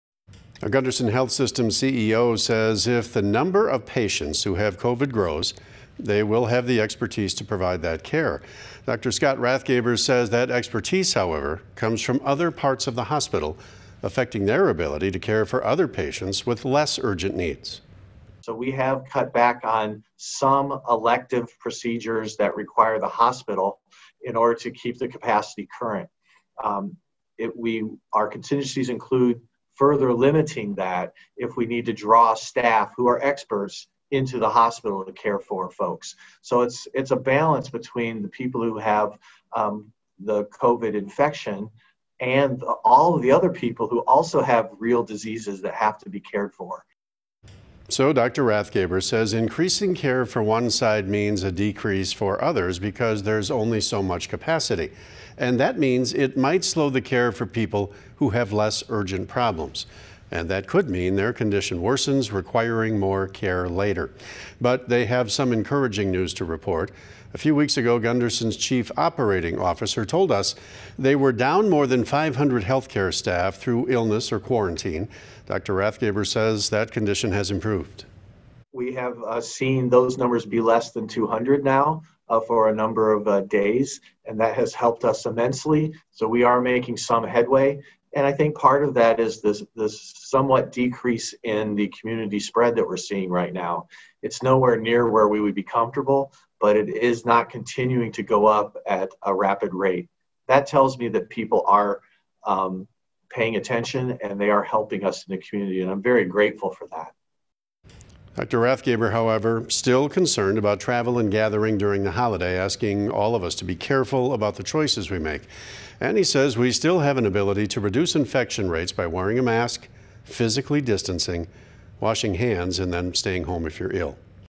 Gundersen committed to providing community care
Gundersen Health System's chief executive officer says if the number of patients with COVID-19 grows, they will have the expertise to provide care.